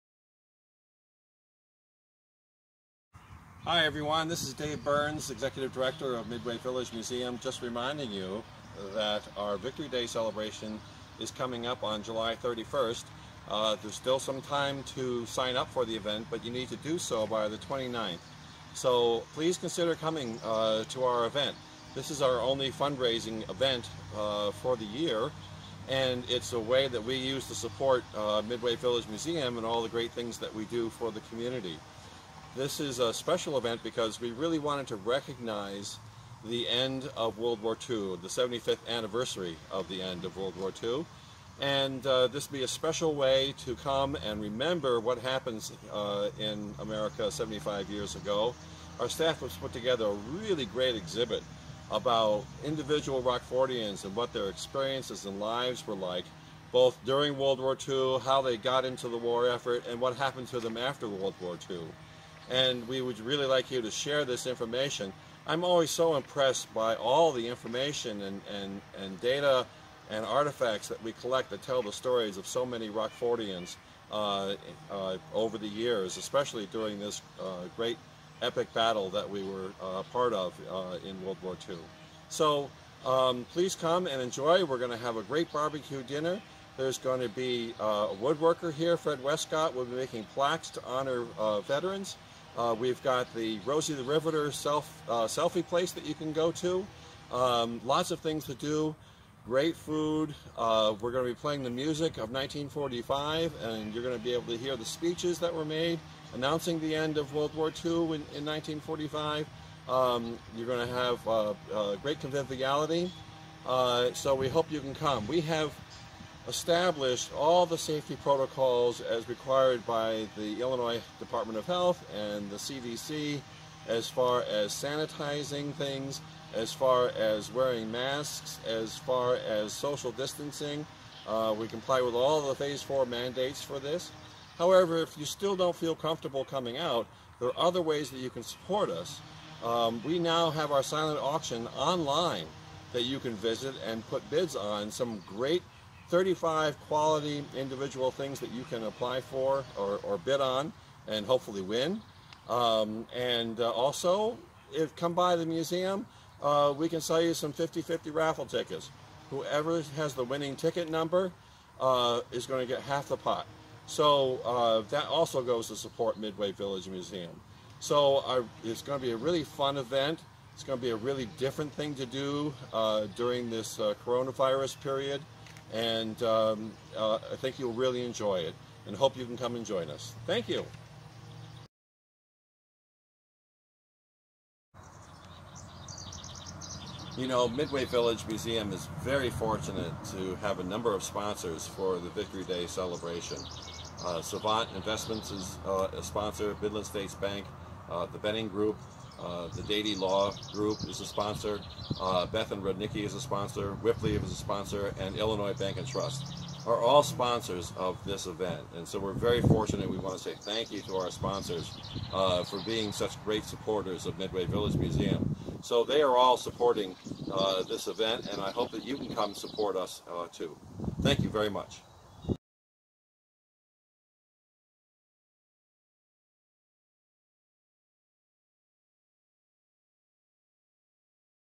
Victory Day Celebration 2020 Annual Fundraiser
The 2020 annual fundraiser at Midway Village Museum is slated for Friday, July 31, as a celebration of the ending of World War II.  The event is outdoors and was planned following the current COVID-19 safety protocols and guidelines.  For compete details about the fundraiser,